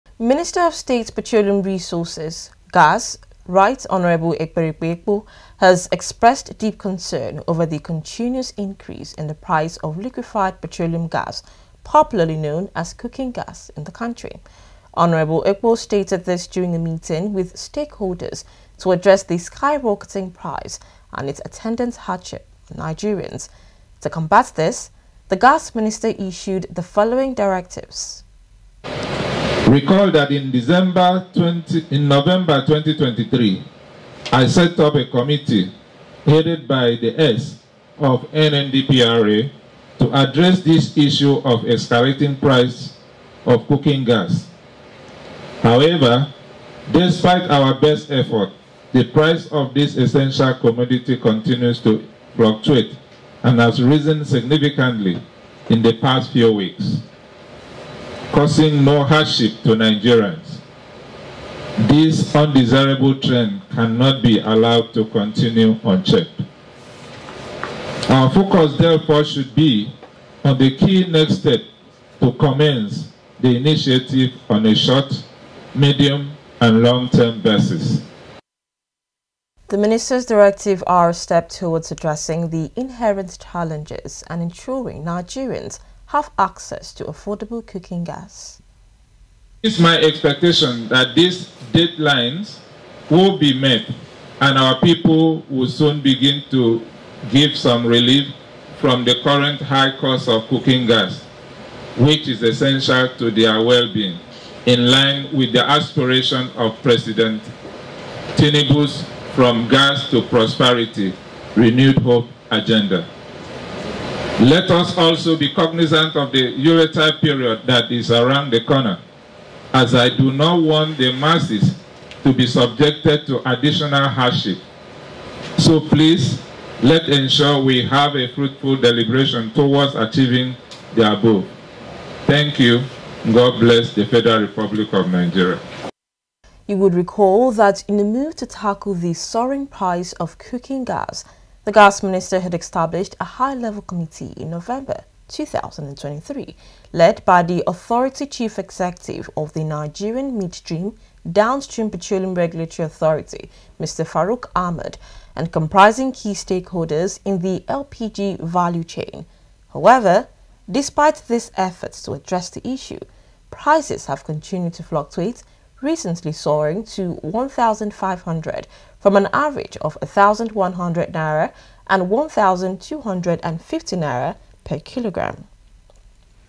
RT. HON EKPERIKPE EKPO CALLS FOR ACTION ON INCREASING COOKING GAS COSTS
Minister of State Petroleum Resources, RT. Hon. Ekperikpe Ekpo has issued directives to reduce the cost of Liquefied Petroleum Gas (LPG), or cooking gas, in the country. The directives aim to ensure affordable cooking gas for Nigerians. Despite the high-level committee established in November 2023, prices have continued to fluctuate, recently reaching N1,500 per kg.